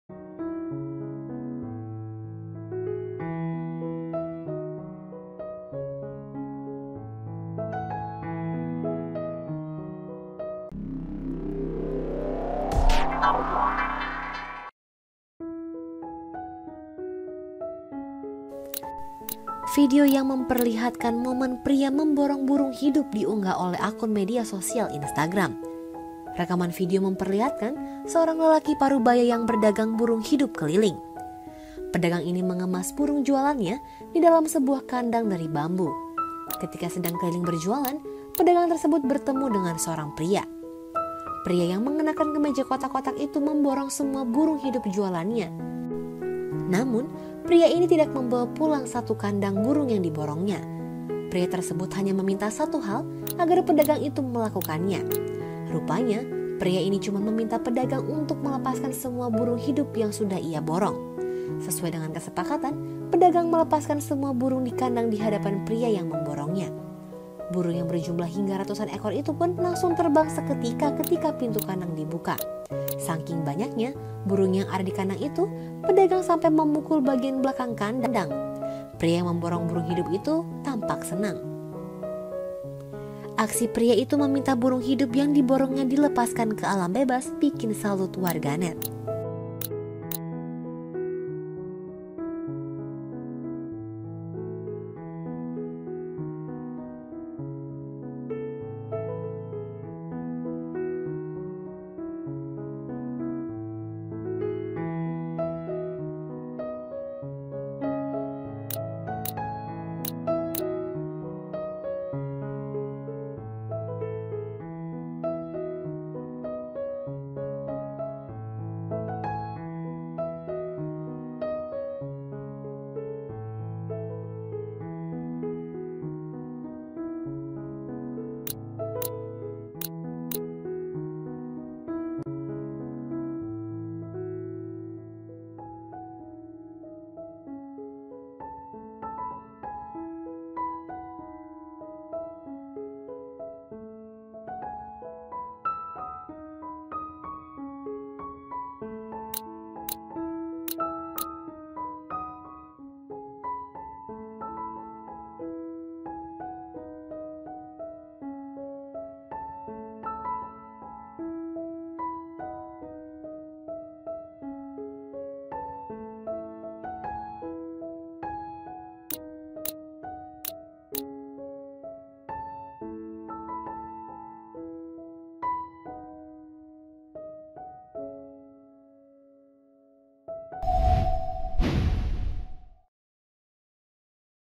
Viral Pria Borong Burung Lalu Dilepaskan
Video Viral Pria Borong Burung Satu Kandang Untuk Dilepas Lagi

Rekaman video memperlihatkan seorang lelaki paruh baya yang berdagang burung hidup keliling.
Pedagang ini mengemas burung jualannya di dalam sebuah kandang dari bambu. Ketika sedang keliling berjualan, pedagang tersebut bertemu dengan seorang pria.
Pria yang memakai kemeja kotak-kotak itu memborong semua burung hidup jualan. Namun, pria ini tidak membawa pulang satu kadang burung yang diborongnya.
Pria tersebut hanya meminta satu hal agar pedagang itu melakukannya. Rupanya pria ini cuma meminta pedagang untuk melepaskan semua burung hidup yang sudah dia borong.
“Bapak ini borong semua burung dan melepaskan nya ke alam bebas,” keterangan video tersebut.
Sesuai dengan kesepakatan, pedagang melepaskan semua burung di kandang dihadapan pria yang memborongnya. Burung yang berjumlah hingga ratusan ekor tersebut langsung terbang ketika pintu kandang dibuka.
Saking banyaknya burung yang ada di kandang itu, pedagang sampai memukul bagian belakang kandang. Pria yang memborong burung hidup dari pedagang itu tampak senang.
Sewaktu burung terbang bebas keluar dari kadang, pria yang memborongnya mengayunkan tangan seolah menginzinkan hewan ini pergi jauh. Pria tersebut terus melihat ke arah burung-burung yang berhasil dibebaskannya.
Aksi pria itu meminta burung hidup yang diborongnya dilepaskan ke alam bebas bikin salut warganet.

VE: d2k
VO: Nadya